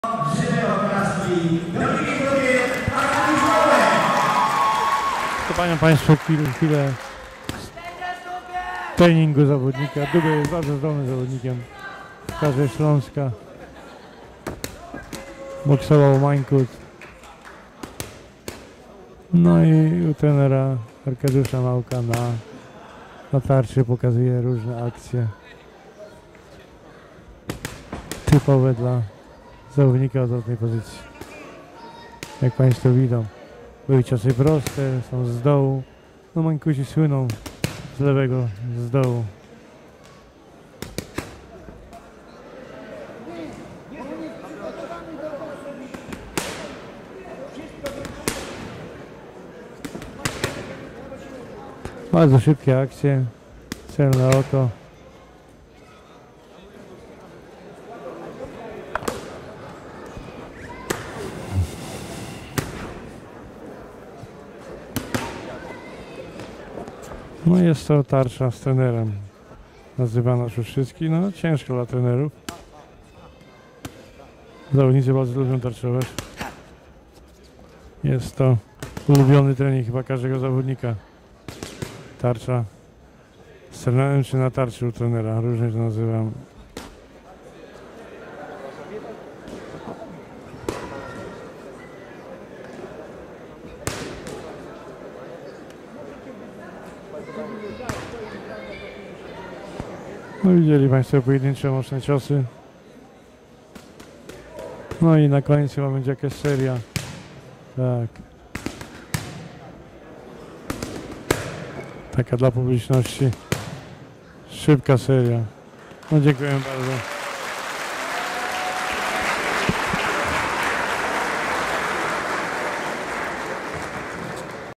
IX MTB im.Kopytka: trening pokazowy - tarcza z trenerem
- Największy portal bokserski w Polsce przedstawia trening pokazowy podczas IX Międzynarodowego Turnieju Bokserskiego im.Roberta Kopytka w Jaworznie